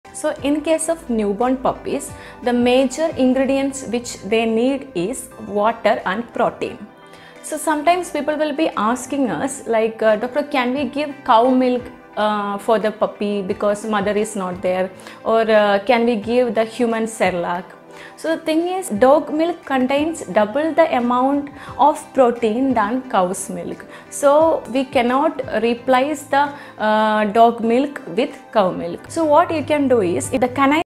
Can I Give Cow Milk to Puppy???? Noooo...... Know why from Dr. Aswathy | Treat Unusual
@Anything About Animals
                     If you have a dog puppy abandoned or have puppies at home but have lost their mother during delivery. Often times puppies fall sick, weak, or lost due to poor information on how to care for the newborn abandoned puppy. Then this is the video from Dr. Aswathy S who explains in detail the care and management of abandoned puppies. She explains How to keep puppies warm, How to Feed them, How to Avoid diseases, How to help them in the elimination of urine or stools in the initial days.

Follow us on